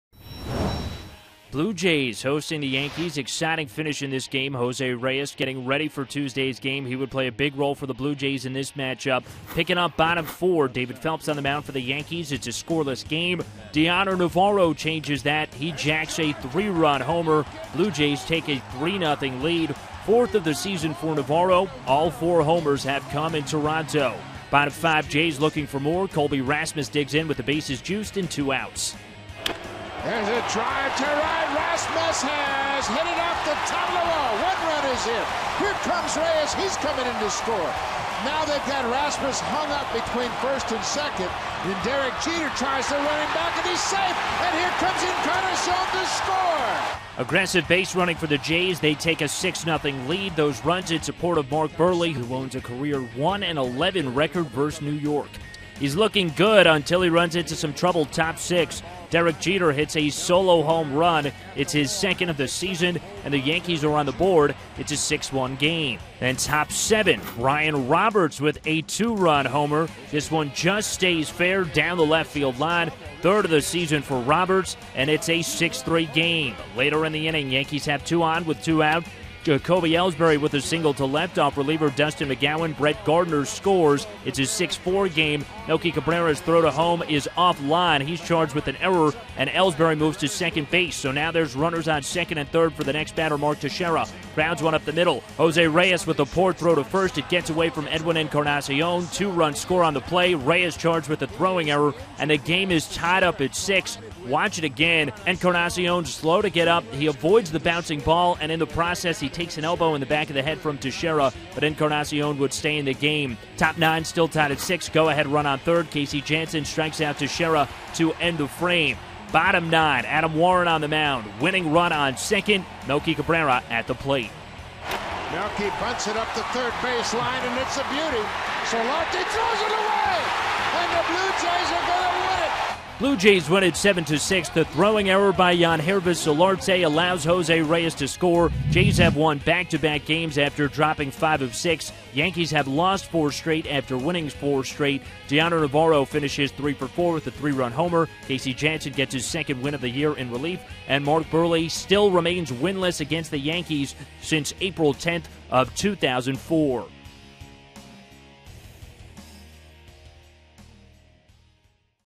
6/24/14: Blue Jays walk off after Yanks rally
Daily Recap: Jose Reyes scored on a Yangervis Solarte error to give the Blue Jays a 7-6 walk-off win despite a furious Yankees' rally